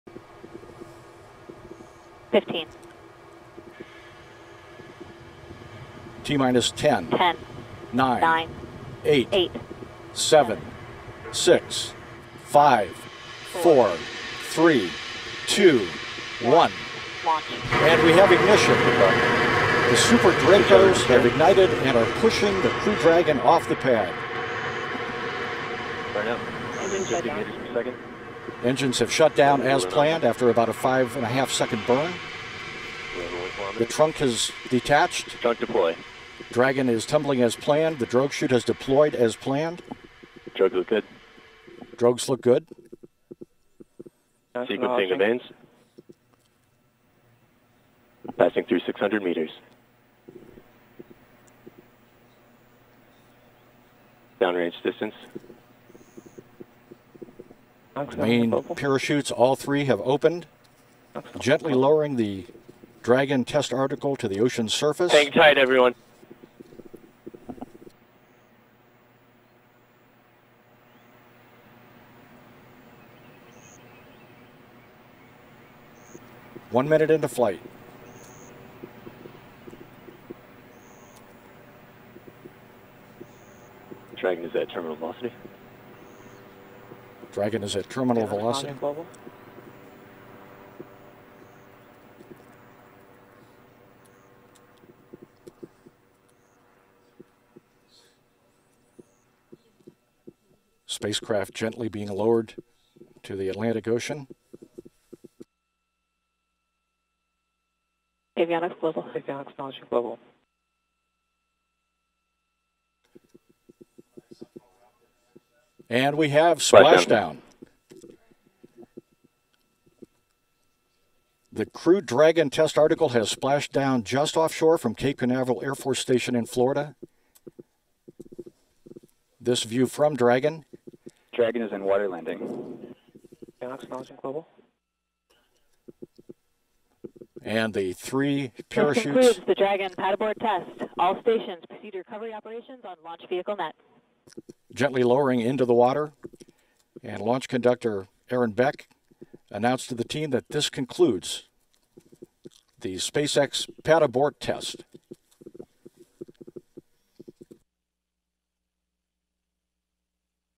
SpaceX Crew Dragon Spacecraft Takes Flight During Pad Abort Test at Cape Canaveral
SpaceX tested its Crew Dragon, a spacecraft under final development and certification through NASA’s Commercial Crew Program (CCP) which simulated an emergency abort from a test stand on Space Launch Complex 40 at Cape Canaveral Air Force Station (CCAFS) in Florida.

The ability to abort from a launch or pad emergency, and safely carry crew members out of harm's way, is a critical element for NASA's next generation of crewed spacecraft. SpaceX will perform the test under its Commercial Crew Integrated Capability (CCiCap) agreement with NASA, but can use the data gathered during the development flight as it continues on the path to certification.  Under a separate Commercial Crew Transportation Capability (CCtCap) contract, NASA's CCP will certify SpaceX’s Crew Dragon, Falcon 9 rocket and ground and mission operations systems to fly crews to and from the International Space Station.